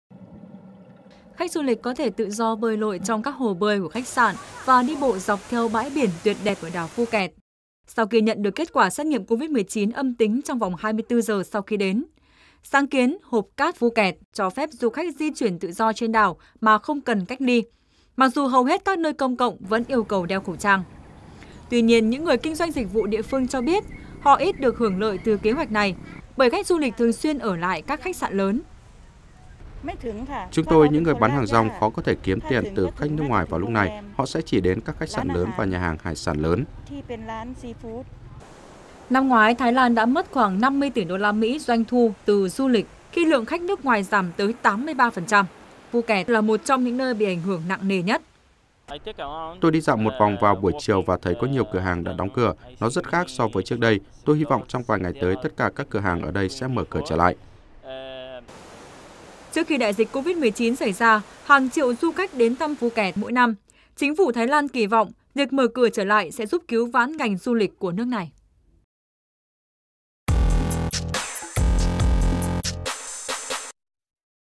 Khách du lịch nước ngoài tận hưởng thời gian du lịch tại đảo Phuket
Khách du lịch nước ngoài mới đến đảo Phuket của Thái Lan đang tận hưởng thời gian nghỉ ngơi thoải mái mà không cần phải cách ly, lần đầu tiên trong hơn 1 năm qua, sau khi Chính phủ Thái Lan tung ra một chương trình đặc biệt dành cho những du khách đã được tiêm phòng đến hòn đảo này.